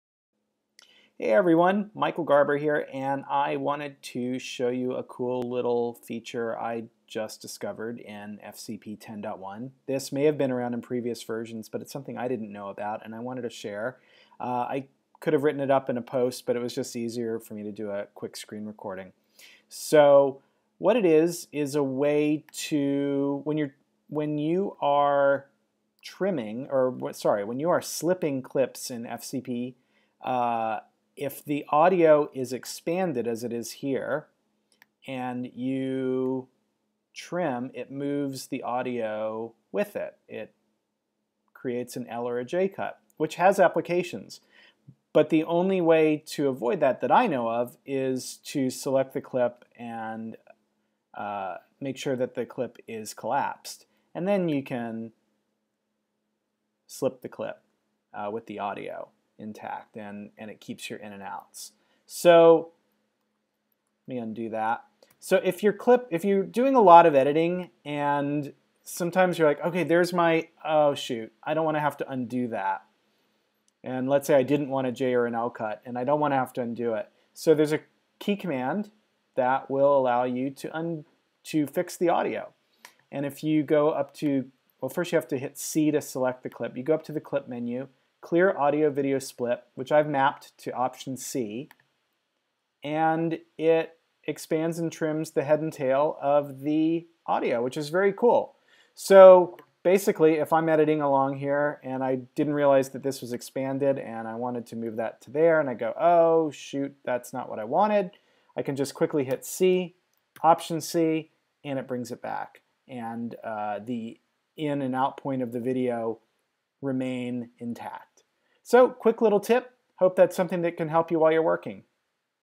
FCP X Quick Tip - Clear Splits
A quick tutorial on clearing splits edits in FCP X.